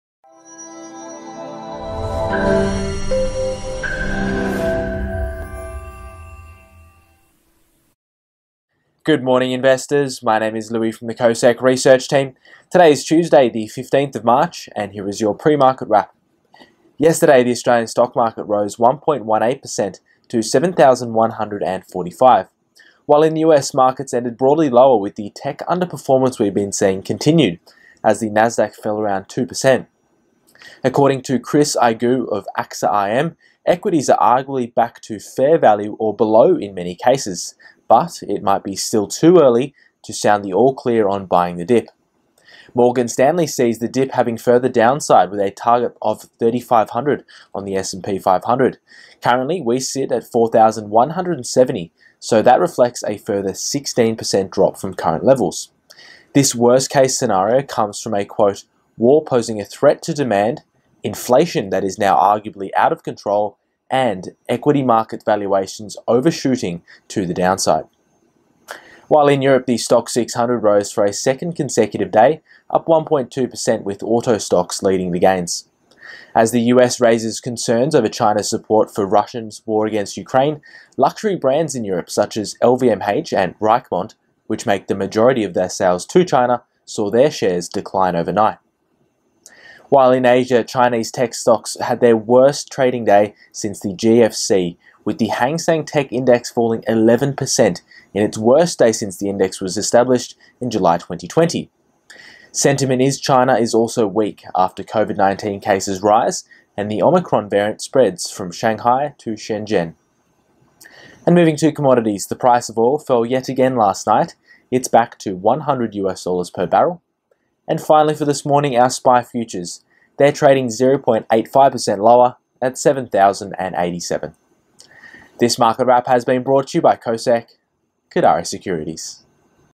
PRE MARKET WRAP 15.03.2022
Good Morning Investors, here is your post-market wrap for 15/03/2022 presented by Research Analyst, Louis Mosmann